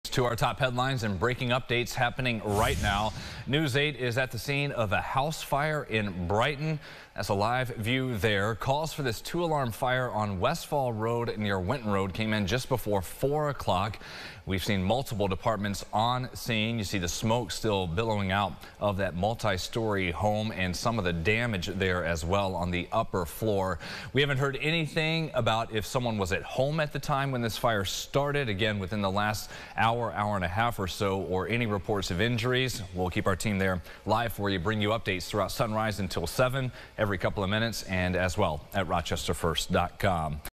Fire crews battle house fire on Westfall Road
Firefighters from multiple agencies are at the scene of a house fire on Westfall Road Friday morning.